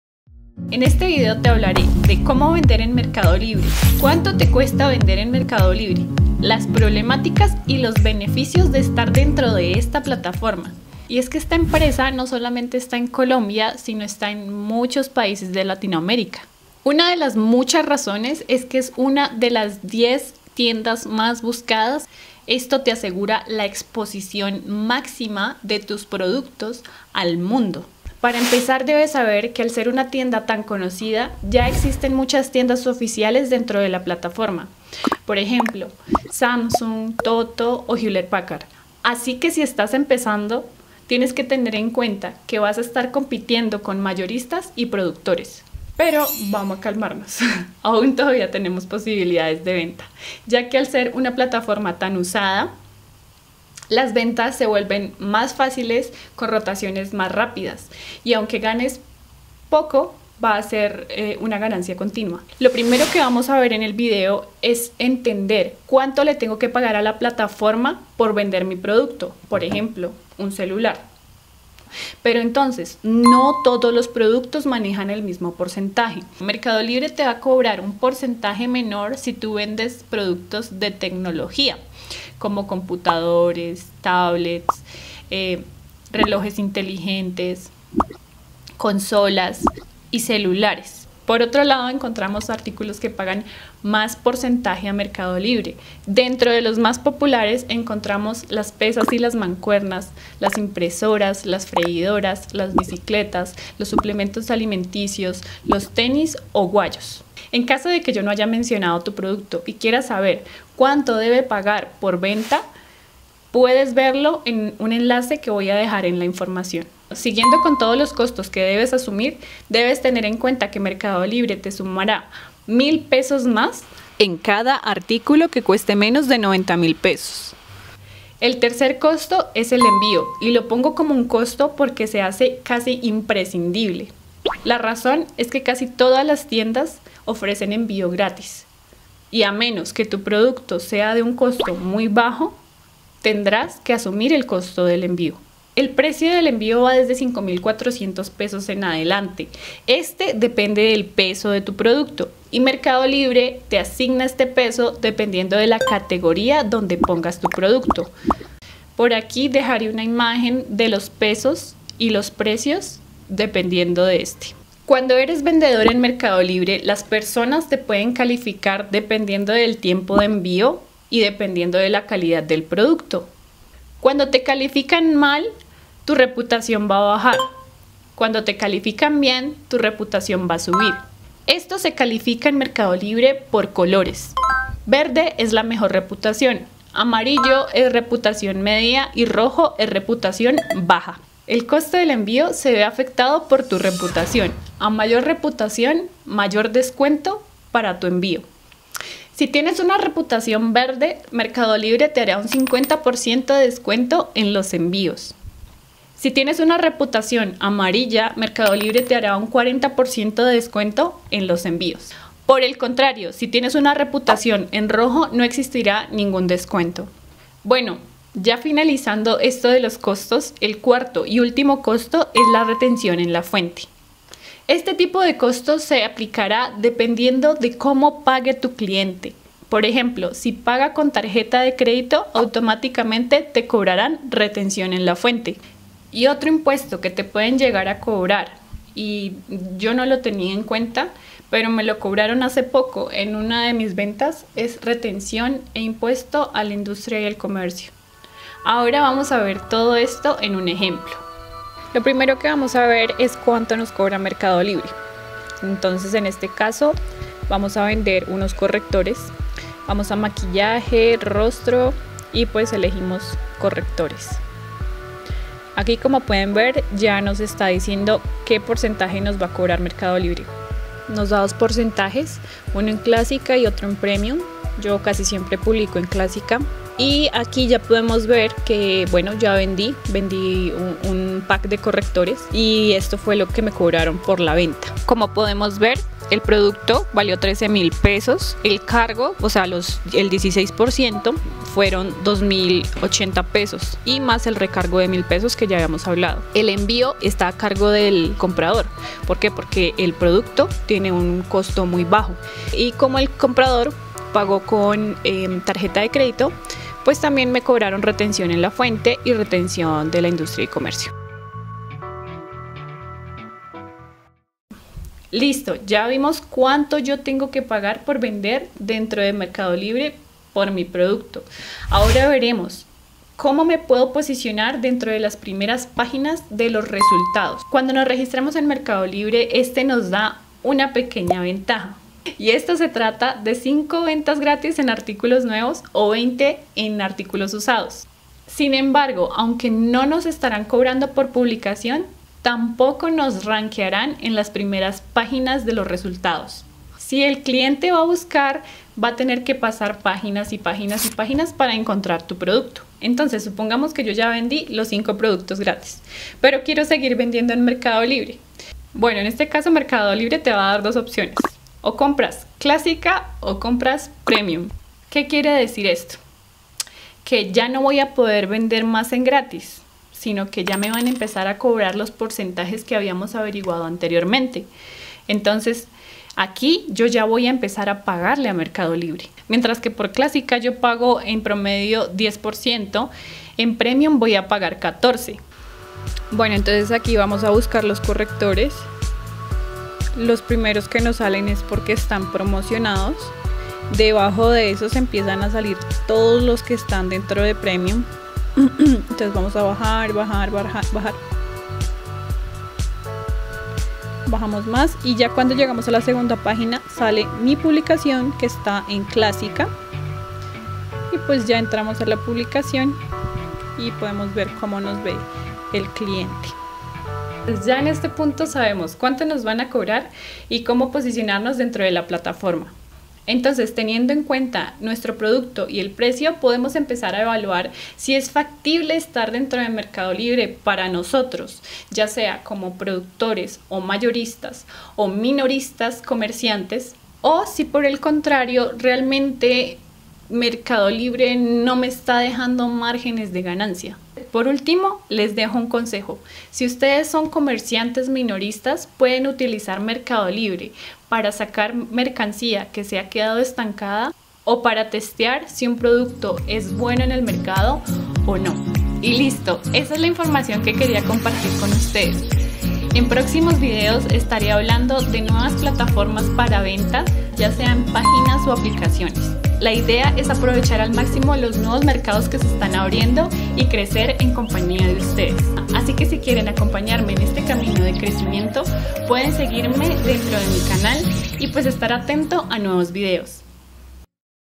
COSTOS COMISIONES Y POSICIONAMIENTO EN MERCADO LIBRE
Con este video podrás saber cuanto te cobrara MercadoLibre antes de publicar cualquier producto, de esta manera entenderás si es bueno o no vender en mercado libre.

00:00 Inicio
00:15 Beneficios de mercado libre
00:29 Problemáticas de mercado libre
01:03 Costos de mercado libre 1 costo
02:00 Costos de mercado libre 2 costo recargo de $1000
02:11 Costos de mercado libre 3 costo , el envió
02:49 El costo del envió se ve afectado por la reputación
03:45 Costos de mercado libre 4 costo , retención en la fuente
04:10 Ejemplo costos de mercado libre
05:20 Posicionamiento en mercado libre
06:24 Ejemplo posicionamiento
06:58 Consejos finales